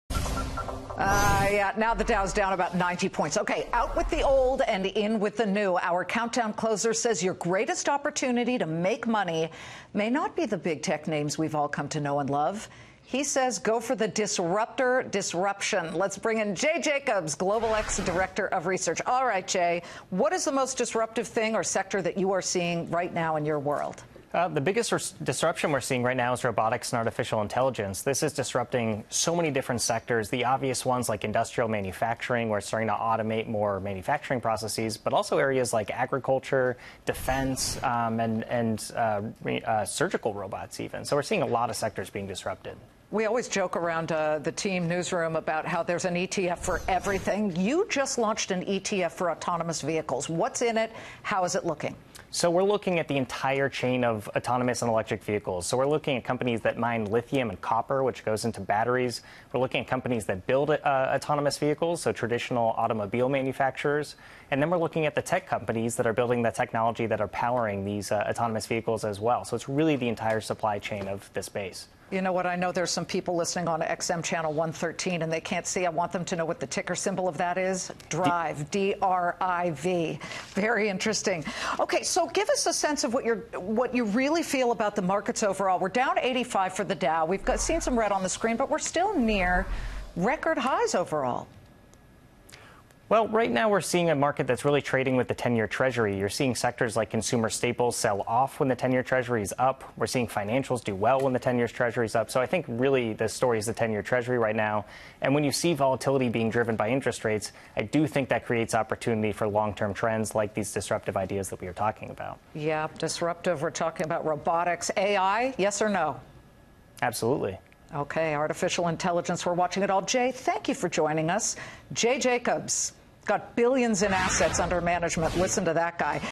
Robotics, artificial intelligence is disrupting a myriad of sectors: Jay Jacobs
Global X Director of Research Jay Jacobs on why to invest in robotics and artificial intelligence.